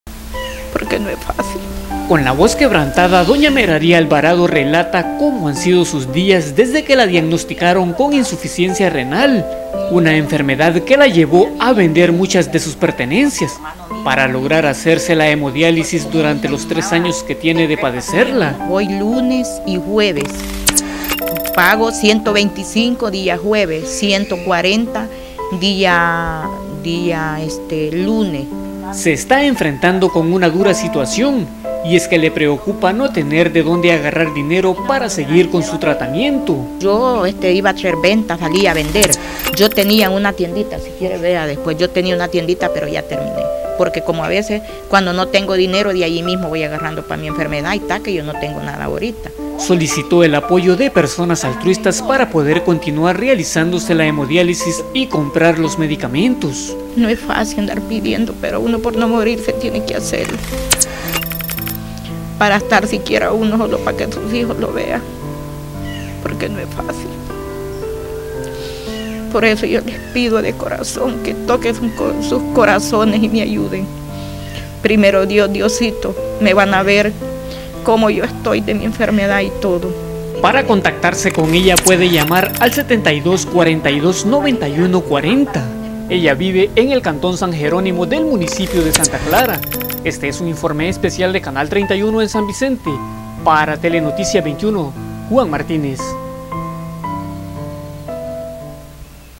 NOTICIA
NOTICIA: Merary Alvarado originaria del cantón San Jerónimo, municipio de Santa Clara padece insuficiencia renal y pide ayuda para seguir con su tratamiento.-